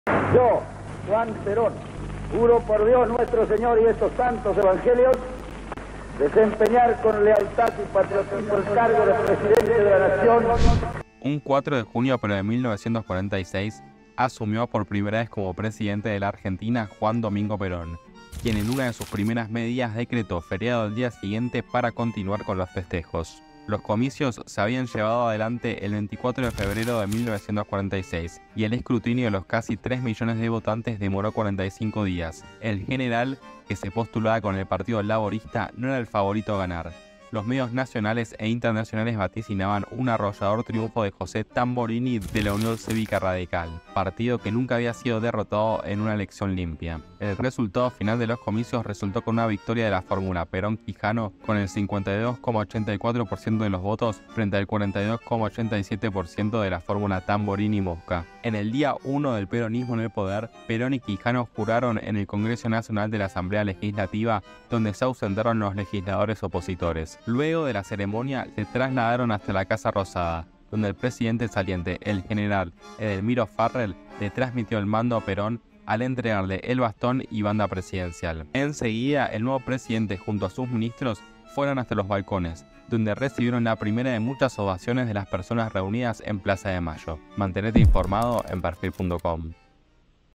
Cómo fue la asunción de Perón en 1946, el “día uno” del peronismo
📌 Hace 78 años asumió la primera de sus tres presidencias el padre del Justicialismo. El General no era el favorito para ganar las elecciones ya que del otro lado se enfrentaba a la Unión Cívica Radical que por entonces era el partido dominante.

🗳️ Fueron las últimas elecciones del voto “universal” de la ley Sáenz Peña, que era sólo para varones, indirecto y por colegios electorales provinciales. Ya en las elecciones de 1951 se aplicaría el sufragio para todos los ciudadanos del país, incluyendo a las mujeres,